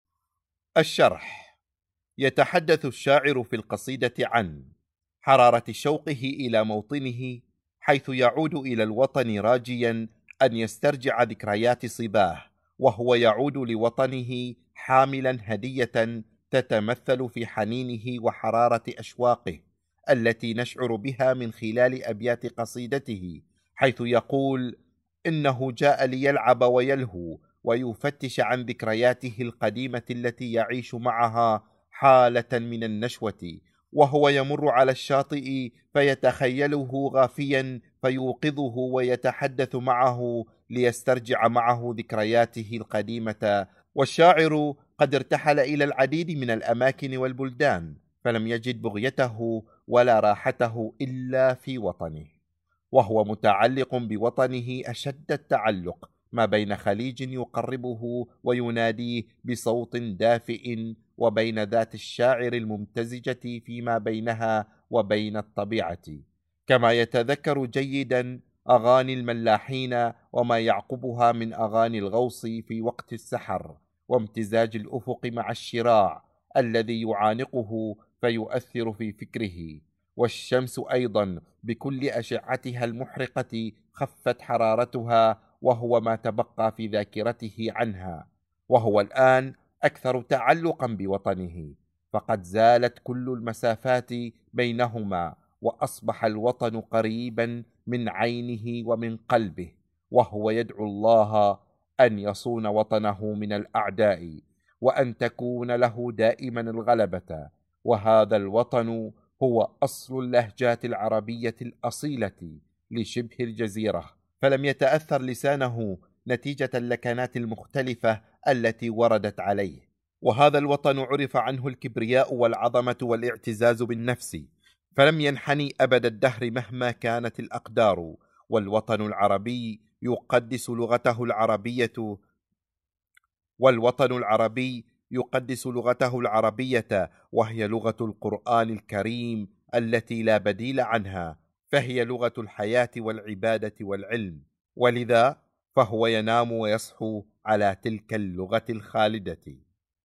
أغنية الخليج لغازي القصيبي- الشرح
اللغة العربية
الصف العاشر
الفصل الدراسي الأول
تابع الدرس: أغنية الخليج- الشرح

ملاحظة_مهمة:- توجد بعض الاخطاء الاملائية
😌يتحدث الشاعر في القصيدة عن حرارة شوفة إلى موطنه حيث
ويعود إلى وطنه راجيا أن يسترجع ذكريات صباه
وهو يعود للوطن هي حامل الهدية تتمثل في حنانه وحرارة أشواقه التي نشعر بها من خلال أبيات قصيدة
حيث يقول  انه جاء يلعب ويلهو ويفتش عن ذكرياته القديمة التي يعيش معها حالة من النشوة
وهو يمر على الشاطئ 🏝فيتخيل غافيا يوقظ وهو يتحدث معه ما يسترجع معهم ذكرياته القديمة
 والشاعر قد أرتحل إلى العديد من الأماكن والبلدان ولم يجد بغيته ولا راحته منه الا في وطنه وهو متعلق بوطنه أشد تعلق ما بين خليج يقربه وناديه بصوت الدافيء وبين ذات الشاعر الممتزج بينها وبين الطبيعة
 كما يتذكر  جيدا أغاني الملاحين وما يعقبها  من أغاني الغوص في وقت السحر وامتزاج الأفق مع الشراع الذي
يعنفها فيؤثر في فكره
والشمس ☀️وأيضا بكل أشعتها المحرقة خفت حرارتها وهو ما تبقى في ذاكرته عنها وهو الآن اكثر وتعلقا في وطنه فقد زالت ت كل المسافات بينهم و أصبح الوطن قريبا من عينه ومن قلبه وهو يدعو الله أن يصون وطنه من الأعداء وأن تكون له دائما غلبك
وهذا الوطن هو اصل  اللهجات العربية الأصيلة شبه الجزيرة ولم يتأثر لسانه نتيجة اللكنات المختلفة التي وردت عليه
وهذا الوطن أعرف عنه والكبرياء والعظم والاعتزاز بالنفس فلم ينحني أبدا الدهر مهما كانت الاقدار والوطن العربي يقدس اللغة العربية
والوطن العربي يقدس اللغة العربية وهي لغة القرآن الكريم التي لها بديل عنها فهيا لغة حياتي والعبادة والعلم
فهو ينام و يصحوا على تلك اللغة الخالدة.